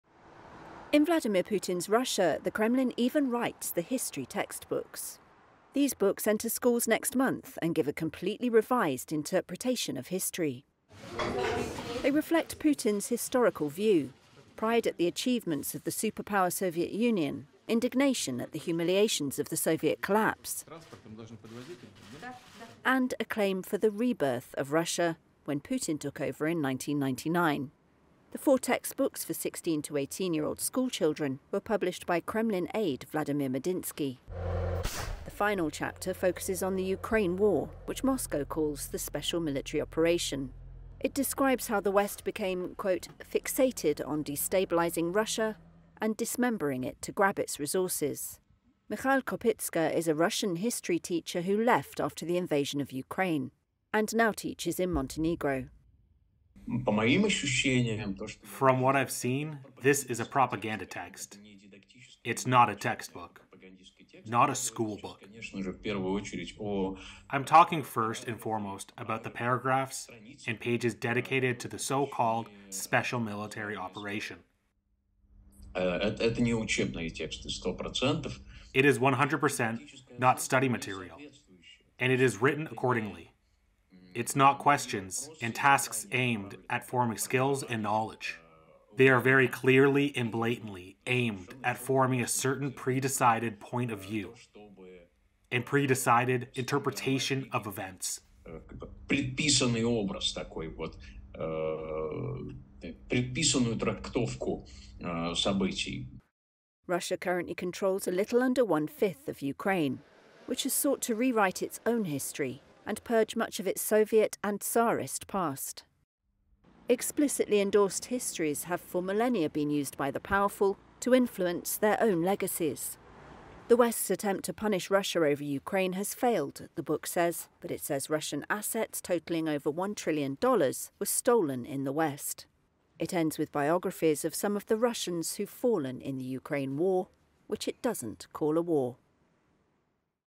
Kremlin aide rewrites history textbooks for a Russia at war
Kremlin aide Vladimir Medinsky has published four history textbooks for 16- to 18-year-old schoolchildren giving a completely revised interpretation of the fall of the Soviet Union, the Vladimir Putin era and the causes of the Ukraine war.